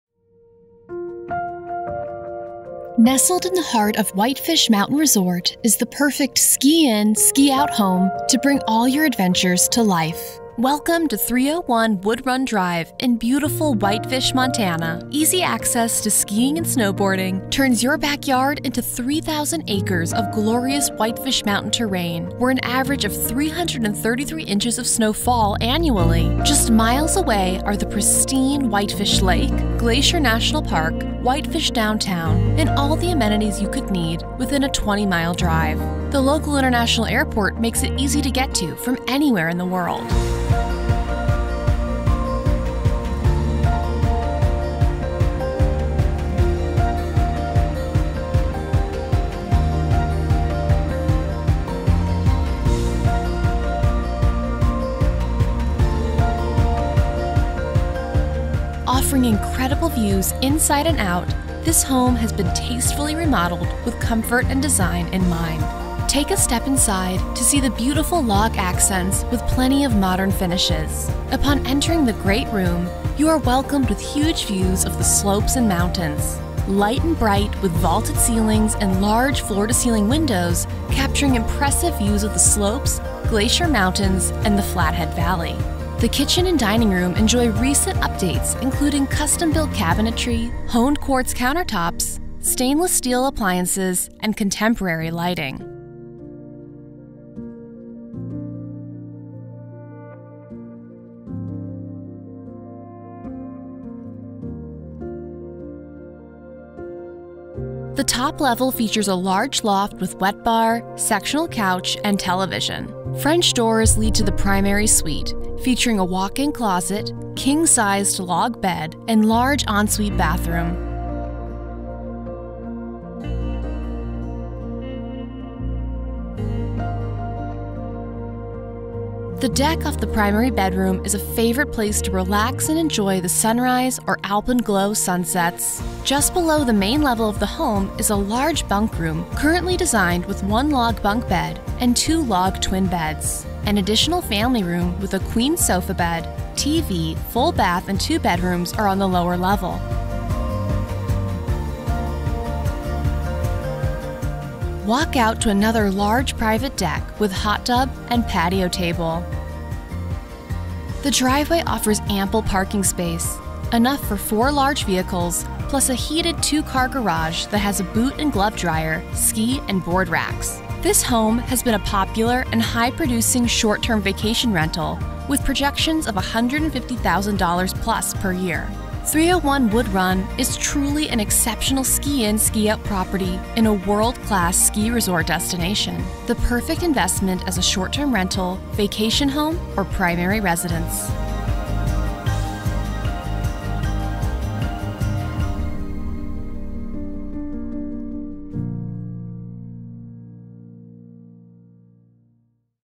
301 Wood Run, Unit D || Whitefish, MT || Listed by Brandon Fairbanks
Exceptional Ski in Ski out property where the views are a ten inside and out. Beautifully remodeled with comfort and design in mind including newer furnishings throughout. The home is light and bright with vaulted ceilings and large floor to ceiling windows capturing impressive views of the slopes. The home features three levels of living space each offering private living areas. On the top level is the primary suite and loft. Complete with custom log furnishings, vaulted ceilings and wonderful covered deck with awe inspiring views from the ski slopes into Glacier and beyond into the valley. A perfect place to begin and end your Montana day. The location is superb in a fully developed neighborhood so your views will never be disrupted by future development. Approximately 200 feet to Home Again allows you to ski right from the driveway.
This home has been a high producing short term vacation rental but has been used as a primary residence since the remodel. All furnishings included. This SKI IN SKI OUT home has been tastefully remodeled with all new flooring, interior paint, custom cabinetry with honed quartz countertops. Most all the furniture is new including the mattresses and bedding.

Upon entering the great room, you are welcomed with huge views of the slopes and mountains. The great room has a river rock gas fireplace. Open to the kitchen and dining room with a large covered deck. The kitchen features stainless steel appliances, custom cabinetry and honed quartz counters. Off the dining room is a large deck with fireplace.
The top level features a large loft with wet bar, sectional couch, and television. Vaulted ceilings and great views make this a wonderful area of the home to enjoy. French doors lead to the primary suite. Featuring a walk-in closet, King sized log bed and vaulted ceilings. The deck off the primary bedroom is one of my favorite places to relax and take in the views.
The primary bathroom has jetted tub, shower, new cabinetry and counter tops.

Just off the main level of the home is a large bunk room currently designed with one log bunk bed and two log twin beds. A very large walk-in owners' closet is off this room.

Continue downstairs and you will find the laundry room with sink, water heater and new furnace. An additional family room with a queen sofa bed, tv, full bath and two bedrooms. Off the lower level is another large deck with hot tub and patio table. The private hot tub accommodates up to 6 adults offering views of the slopes and is much appreciated after a day on the slopes.

The driveway offers ample parking area enough for four large vehicles which is rare to find in this area. The two-car garage is heated and has a boot and glove dryer, plus additional attic storage space.

The home has a new forced air furnace, all new appliances, newer roof, new interior flooring and paint, new lighting and plumbing fixtures throughout. The home is being sold completely furnished including newer kitchenware and custom art work.

The three levels of decking are all in great shape as it was all repaired or replaced this year adding awesome outdoor living spaces.

The home is equipped with surround sound in most rooms including the outside decks. The stereo equipment is all included.

Wood Run Drive is a fully developed neighborhood assuring no future development will obstruct your views. 301 Wood Run offers an exceptional location being insulated from the traffic on Big Mountain Road. The back of the home sits well above Wood Run Drive so noise is minimal, and you don't see any cars. Home Again Ski trail is just a couple hundred feet from the home so skiing to and from your driveway is possible.

This area is zoned allowing short term rentals. Although this home is not currently used as a short-term rental, in the past is has been a very popular and high producing rental.

You can hike and bike right from the home. The Whitefish Legacy Trail can be hiked or biked all the way to town or enjoy hiking and biking on the mountain, picking huckleberries, or take a chairlift ride to the Summit to enjoy a great meal.

301 Wood Run is truly an exceptional property in a very special resort community in Montana.

Seller is a licensed agent in Montana.